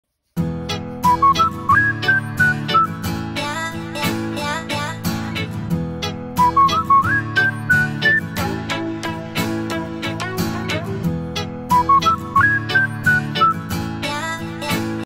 French Bulldog puppy loves Ear Massage
Guilly loves any massage like this ear massage, then he will fall asleep 😊